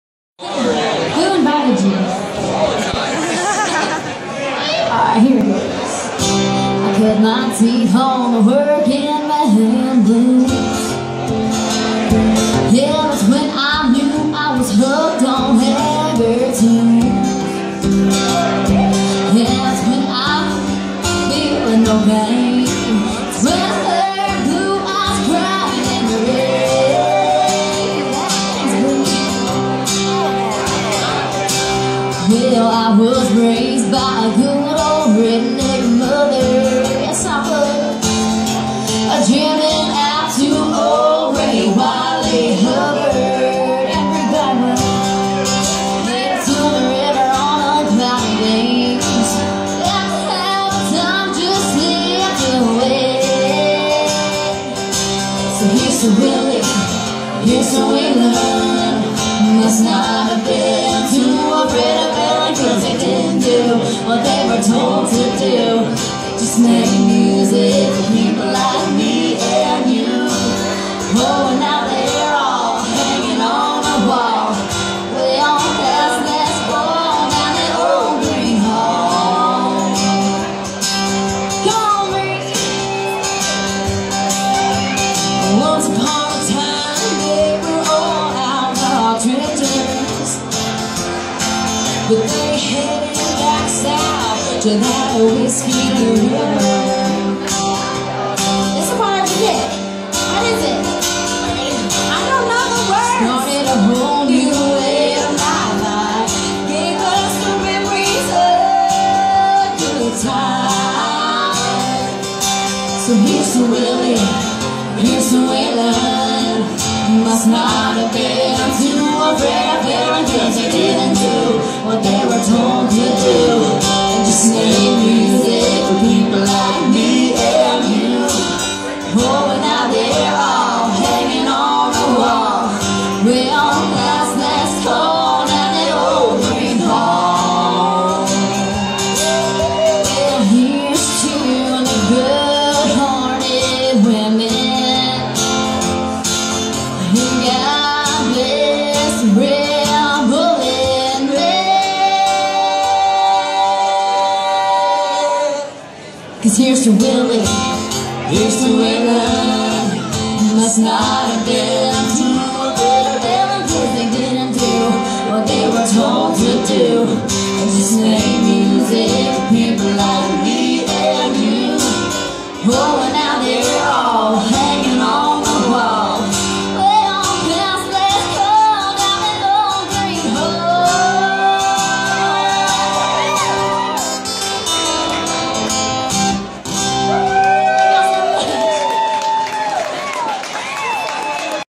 Kenna Danielle, Bri Bagwell & Lyndon Hughes Ole Gruene Hall Steamboat Jan, 2015
Kenna Danielle, Bri Bagwell, Lyndon Hughes